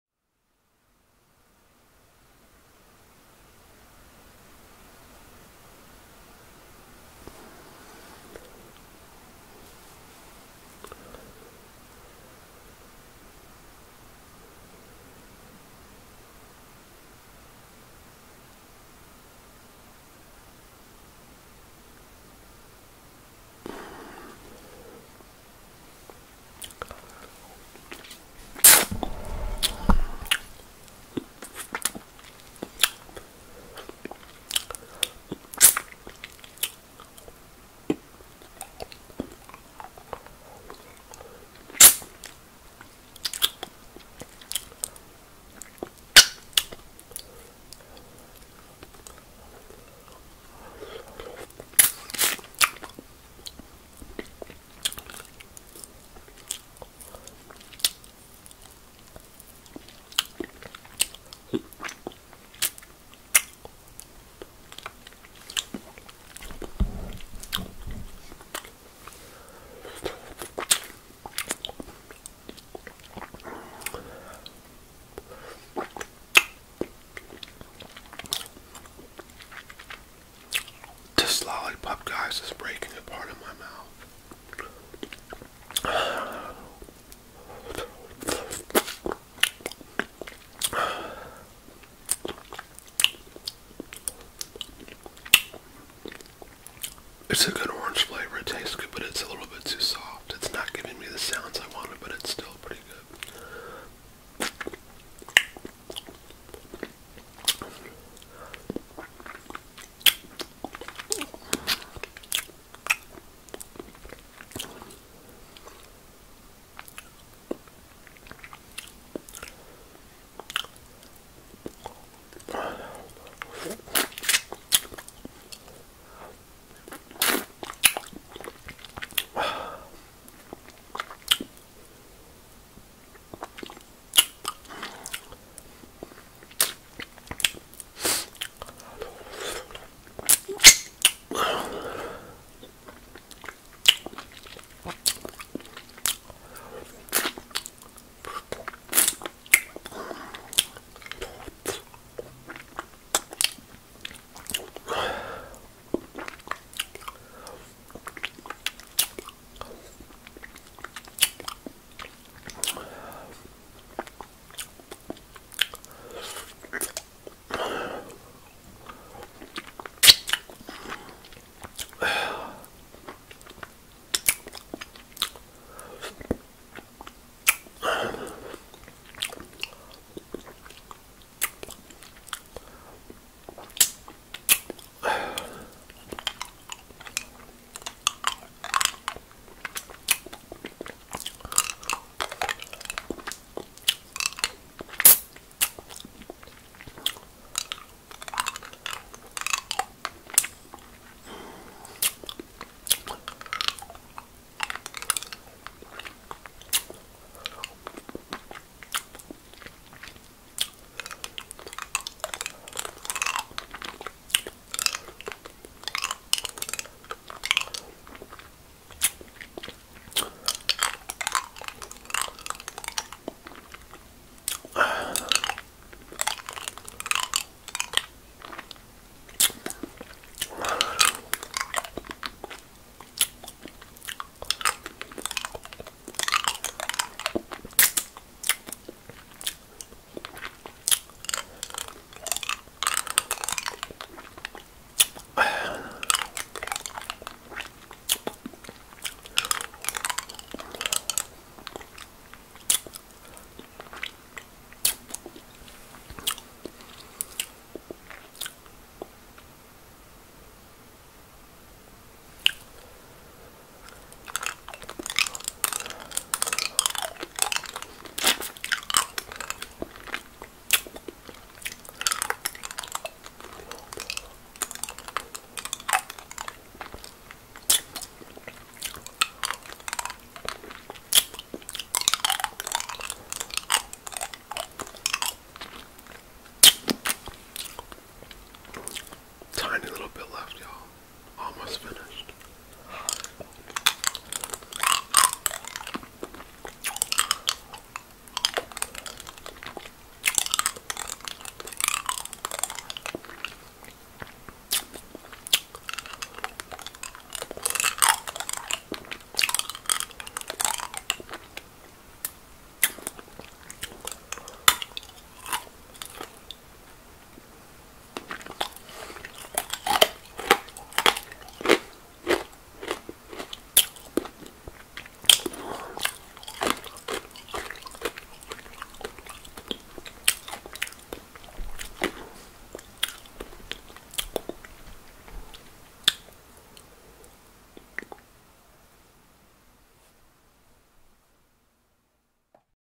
ASMR LOLLIPOP #20. 🍭 SUCKING,LICKING,TEETH SOUNDS,CHEWING.
ASMR Lollipop Eating Sounds. Relaxing. ** Some Talking** 🎧 Plug In For Best Sounds. Make My Day! Enjoy It.💤💤